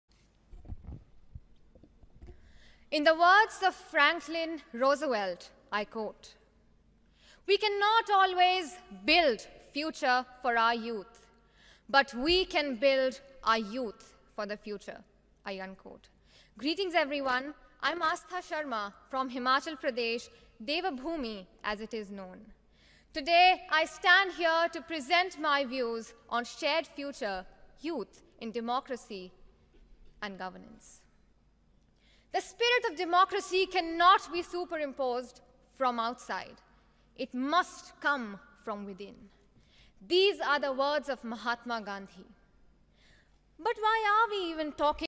Aastha Sharma from NSS unit GC Sanjauli got First Position in NYPF INDIA . PROUD MOMENT FOR HIMACHAL
Aastha Sharma from NSS unit Centre of Excellence Government College Sanjauli, SHIMLA

First Position in NYPF INDIA

PROUD MOMENT FOR HIMACHAL😍❤️